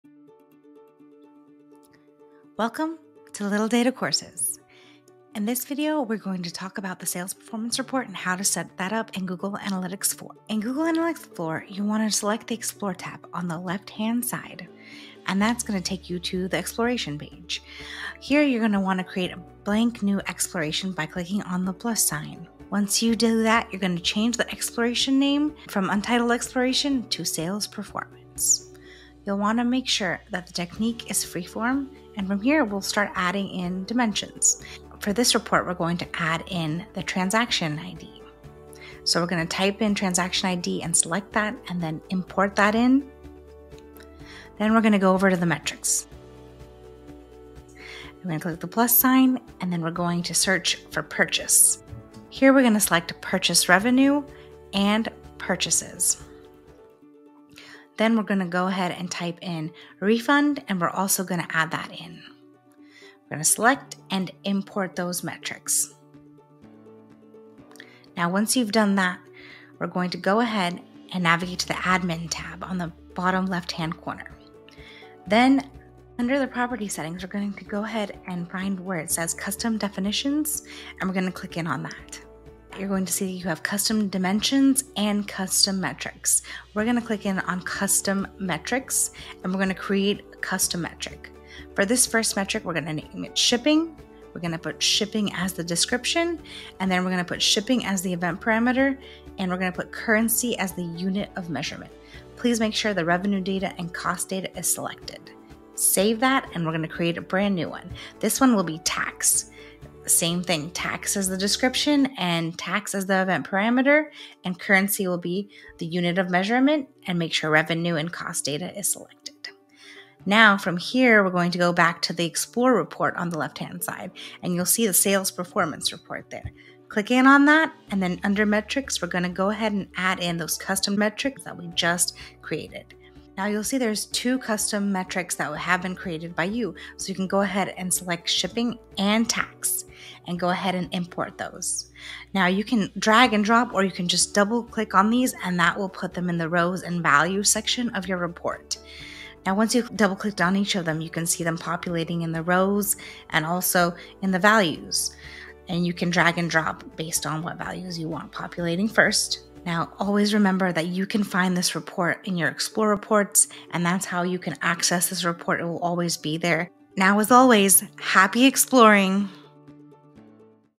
Google Analytics 4 — Sales Performance Report (2023)
Learn how to set up the Sales Performance Report in Google Analytics with updated information based on settings that have changed.

1. Navigate to the Explore tab. From the GA4 homepage, click on the Explore tab on the left-hand side.

2. Start a new exploration. Click the plus sign to create a Blank new exploration.

3. Name the report. Change the exploration name from "Untitled exploration" to Sales Performance. Ensure the technique is set to Free Form.

4. Add dimensions and metrics.

 - Under Dimensions, click the plus sign and search for Transaction ID. Select it and click Import.
 - Under Metrics, click the plus sign and search for the following, then select them: Purchases, Purchase Revenue, and Refunds. Click Import.

5. Create custom metrics.
 - Navigate to the Admin tab (bottom-left corner).
 - Under "Property settings," find and click on Custom definitions.
 - Go to the Custom metrics tab and click Create custom metric.
 - For the first metric:
   - Metric name: Shipping
   - Description: Shipping
   - Event parameter: shipping
   - Unit of measurement: Currency
   - Ensure "Revenue data" and "Cost data" are selected.
   - Click Save.

 - For the second metric:
   - Click Create custom metric again.
   - Metric name: Tax
   - Description: Tax
   - Event parameter: tax
   - Click Save.

6. Add the custom metrics to your report.
 - Go back to the Explore tab on the left.
 - Click on the Sales Performance report you created.
 - Under Metrics, click the plus sign. You will now see the custom metrics you created.
 - Search for and select Shipping and Tax, then click Import.

Organize your report. Drag and drop the dimensions and metrics you've imported into the Rows and Values sections to build your report. You can double-click on each item to add them to the table.

Join 6,000+ DTC marketers who have zero waste on their ad spend and track every conversion for their DTC brands